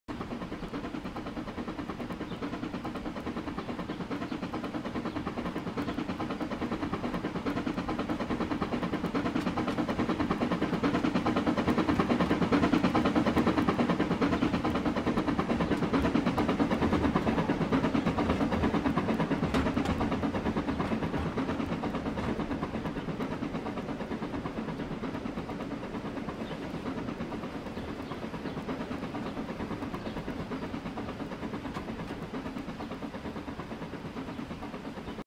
Kitbash - Southern Pacific Daylight #4449
A lil' kitbash I've made of 4449. And no, I won't be distributing this.